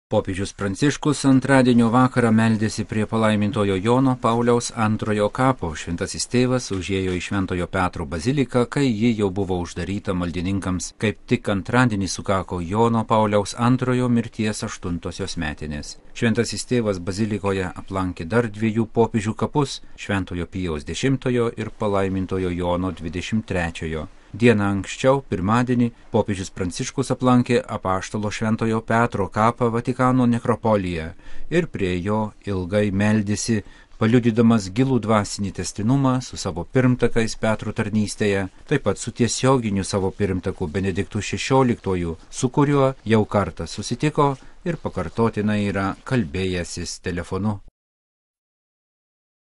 Popiežiaus Pranciškaus malda prie Jono Pauliaus II kapo
Popiežius Pranciškus antradienio vakarą meldėsi prie pal. Jono Pauliaus II kapo. Šventasis Tėvas užėjo į Šv. Petro baziliką kai ji jau buvo uždaryta maldininkams. Kaip tik antradienį sukako Jono Pauliaus II mirties aštuntosios metinės. Šventasis Tėvas bazilikoje aplankė dar dviejų popiežių kapus: Šv. Pijaus X ir pal. Jono XXIII. Dieną ankščiau, pirmadienį, popiežius Pranciškus aplankė apaštalo šv. Petro kapą Vatikano nekropolyje ir prie jo ilgai meldėsi, paliudydamas gilų dvasinį tęstinumą su savo pirmtakais Petro tarnystėje, taip pat su tiesioginiu savo pirmtaku Benediktu XVI, su kuriuo jau kartą susitiko ir pakartotinai yra kalbėjęsis telefonu.